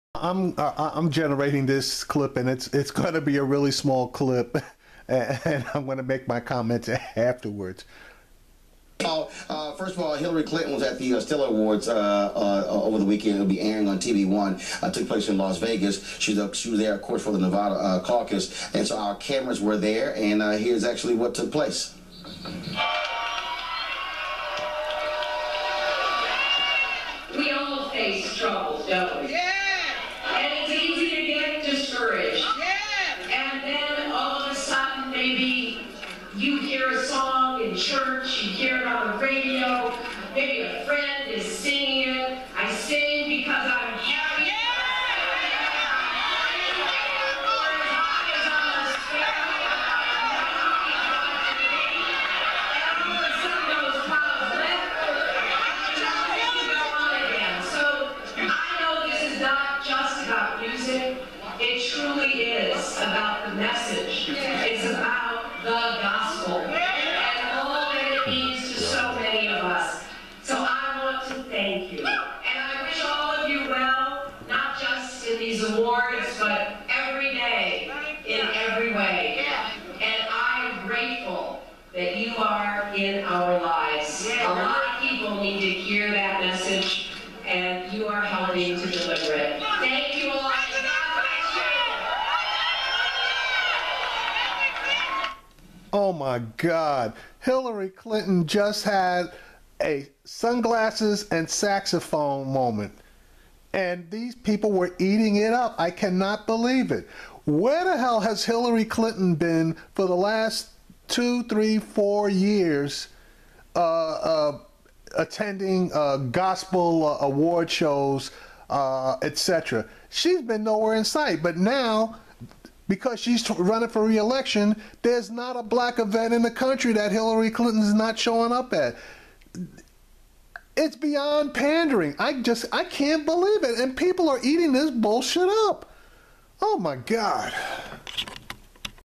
Hillary Clinton's Sunglasses and Saxophone Moment
You can see Hillary Clinton at every major black awards function or event that is being held. Where was she 8 years ago until the past 4 months? No where to be found. Her latest appearance reminds me of Bill's appearance on the Arsenio Hall Show with the sunglasses and saxophone. Does the word disingenuous ring a bell.